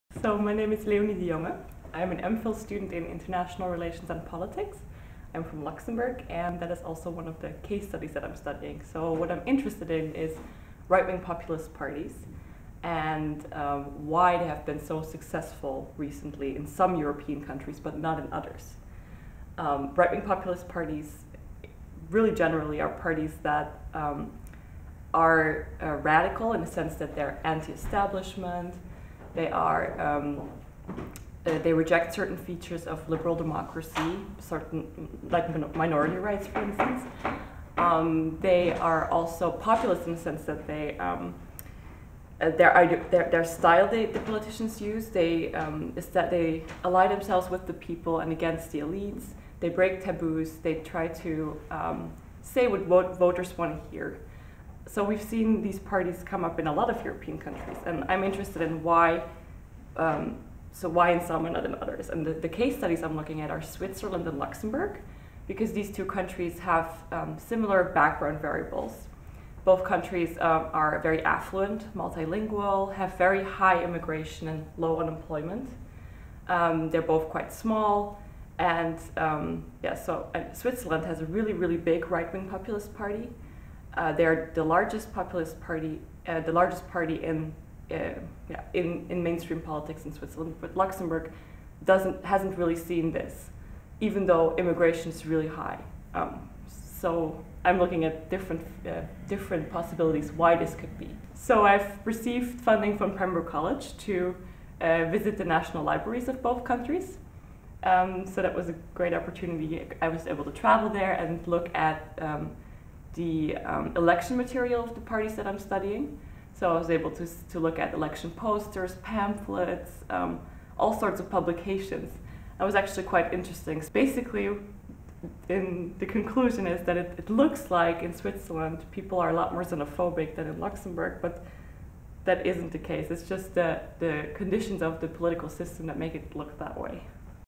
Graduate research: Leonie
Leonie talks about her work to understand the emergence of right-wing populist political parties in Europe